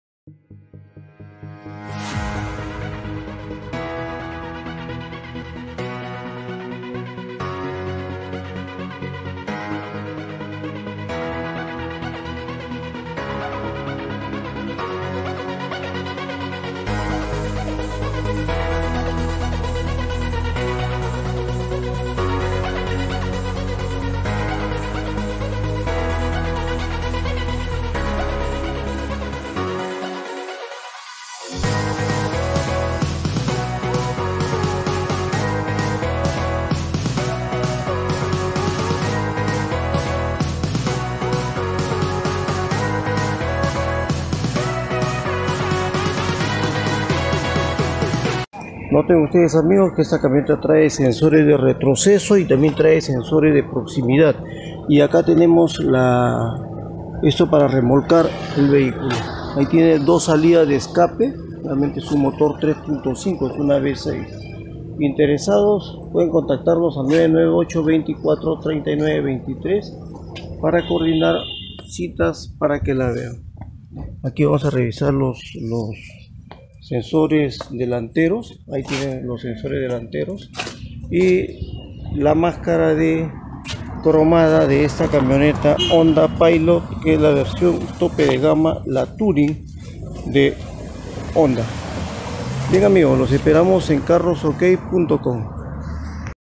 comprenyvendanlomejor-Honda Pilot Touring 2012
Honda Pilot Touring 2012-sensores de retroceso, de proximidad, gancho de remolque y más